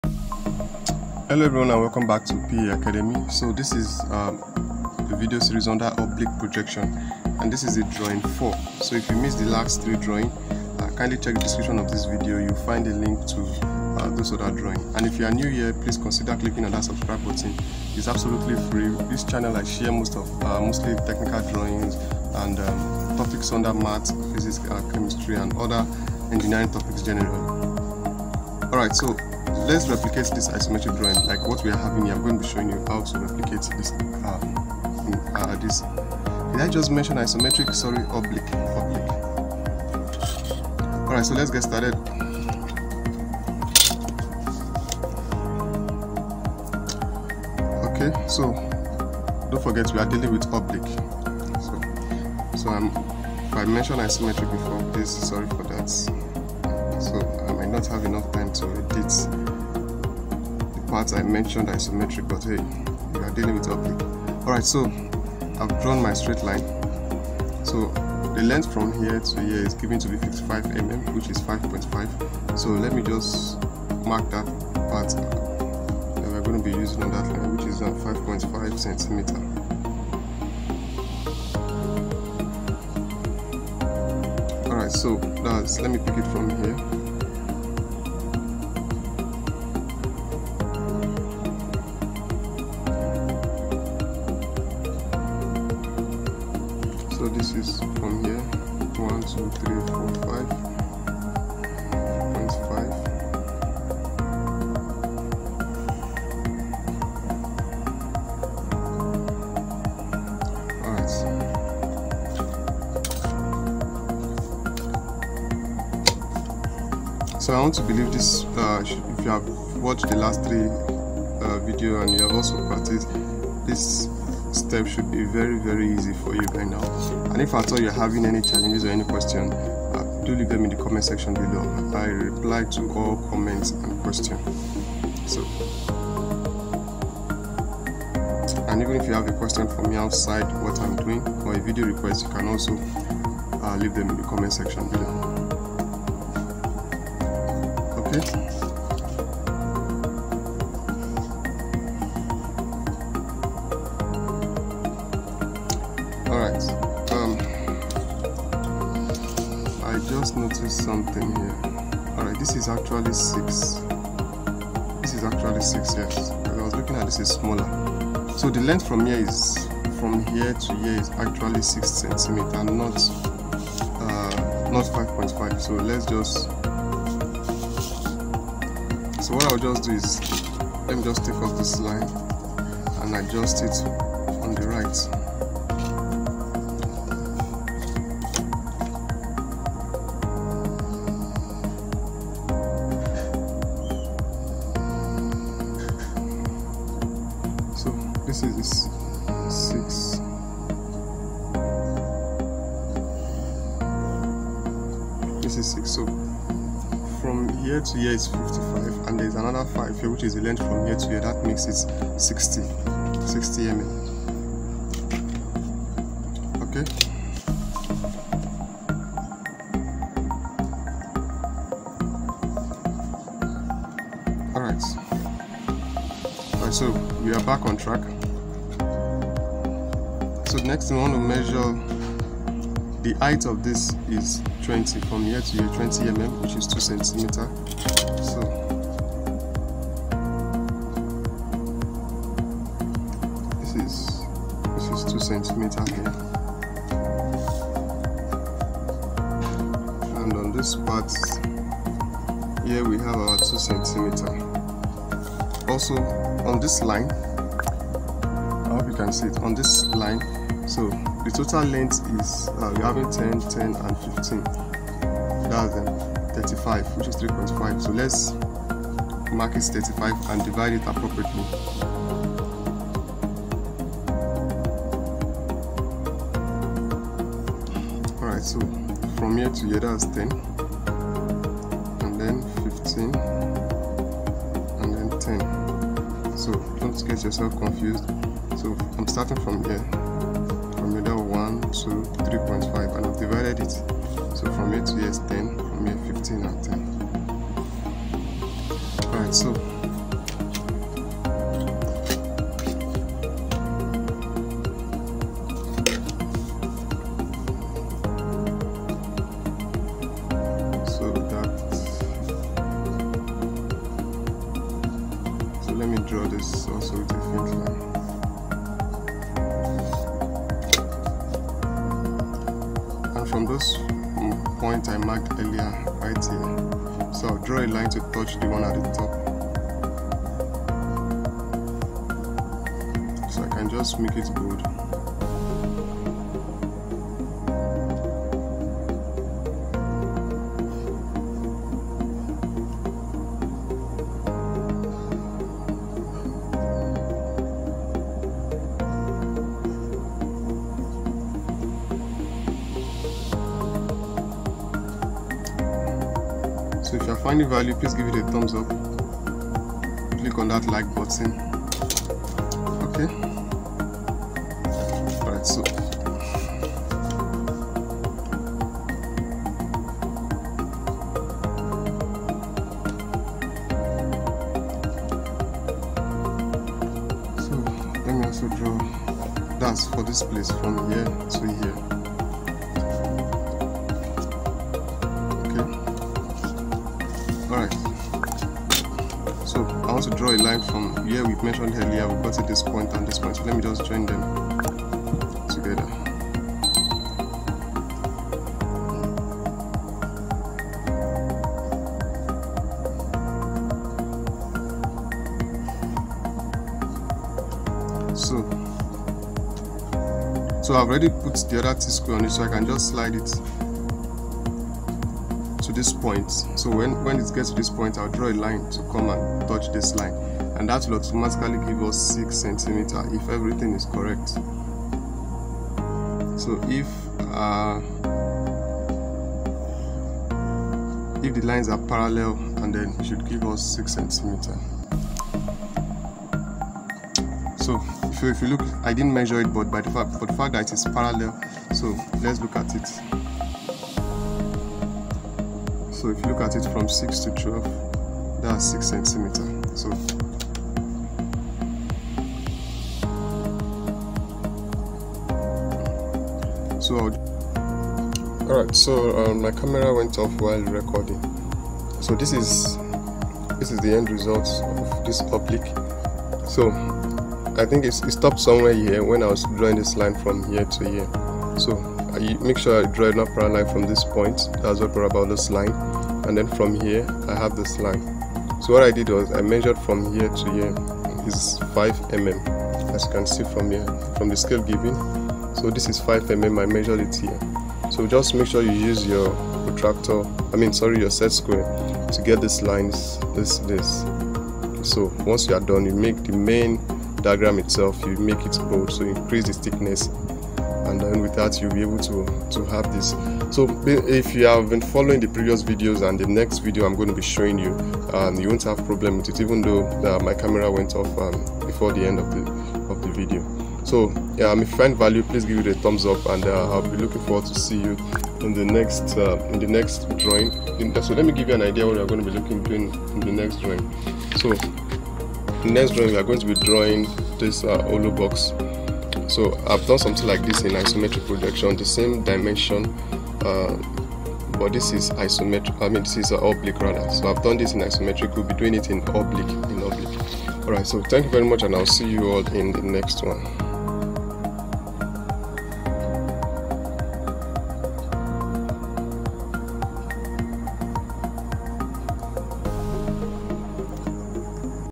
Complex Oblique Drawing Tutorial for Beginners. How to Draw Pictorial View of Object in Oblique
In this video, I teach you all you need to know about oblique projection. I'll cover all the basics of oblique drawing for engineering and technical drawing students.

If you are a beginner in oblique drawing and you need a comprehensive oblique projection tutorial, you're in the right place.

This video series consists of 7 drawings including an Introduction to oblique drawing.

Thanks
PA Academy